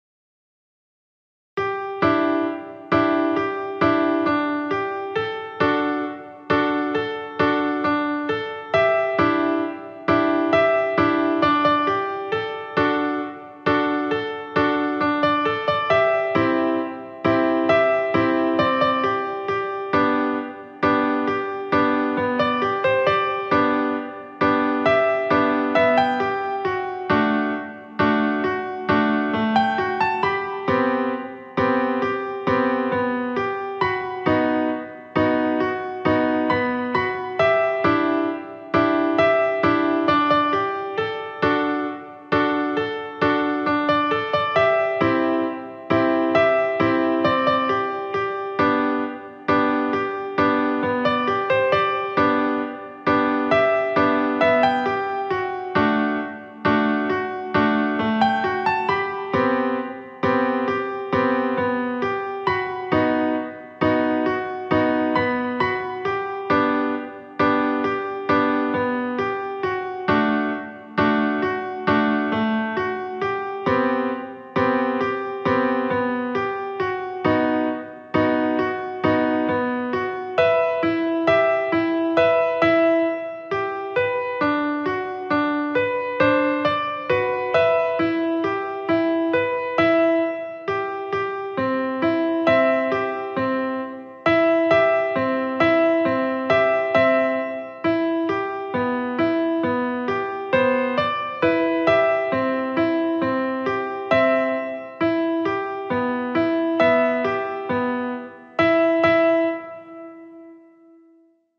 ESTO LO COMPUSO UNA INTELIGENCIA ARTIFICIAL 😱
I asked an AI to compose a piano piece and this was the result.